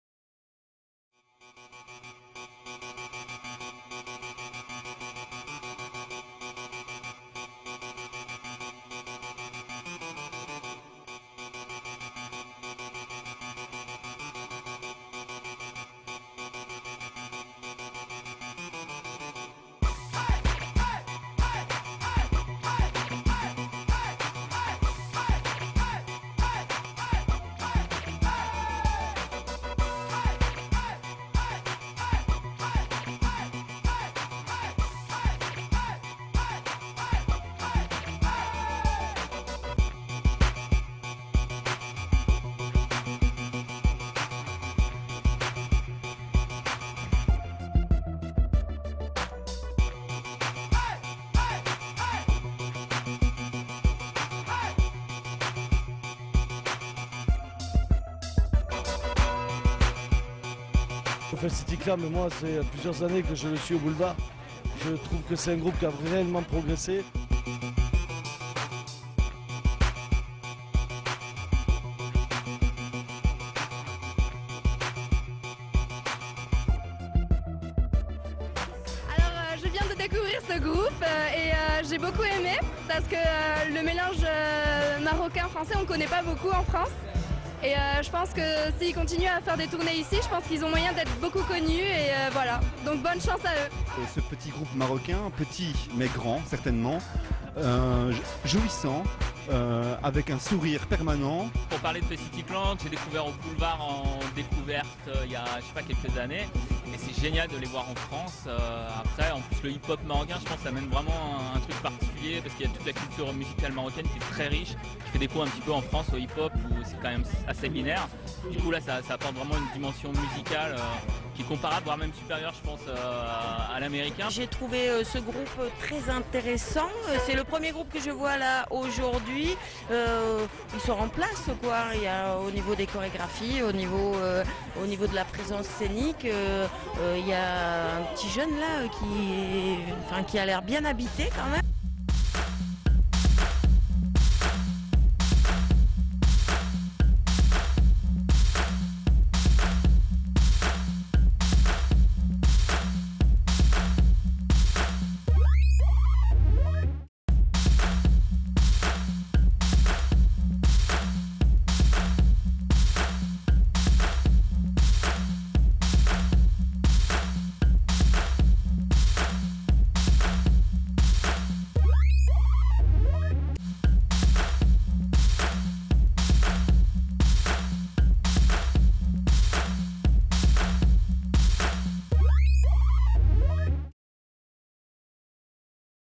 Fez City Clan TeaSer
FEZ CITY CLAN
de retour avec un petit teaser avant la sortie du 1er single de l'album pour le grand plaisir de nos fans qui se demandent souvent pourquoi les fez city clan ont mis beaucoup de temps pour faire leur comback bein voila un petit extrait